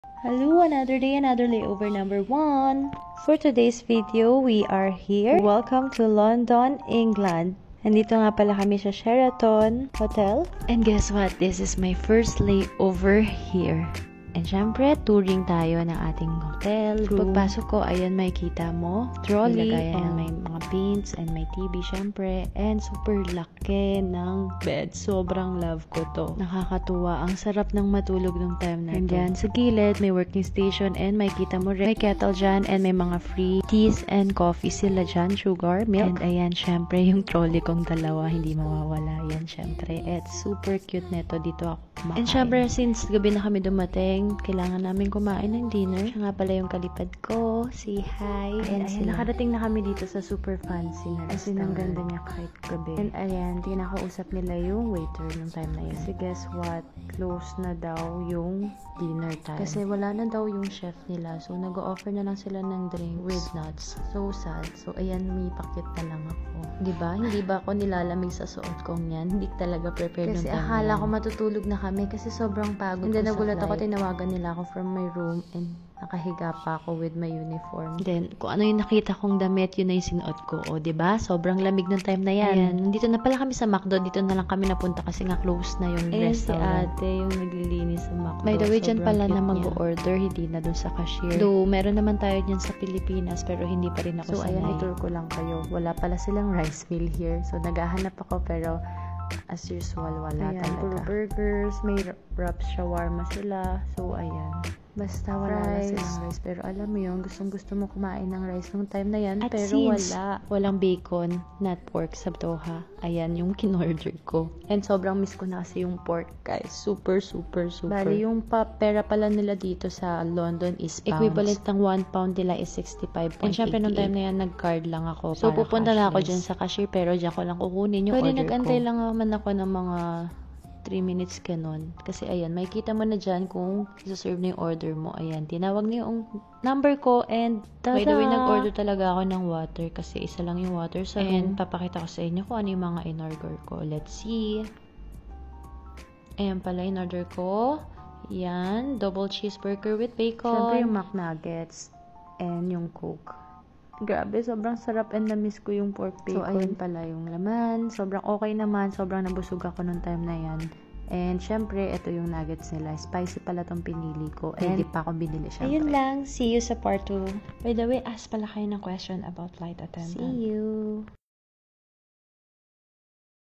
London Layover for 24H🇬🇧
This is my 1st layover experience in LONDON, 🇬🇧 as a Flight attendant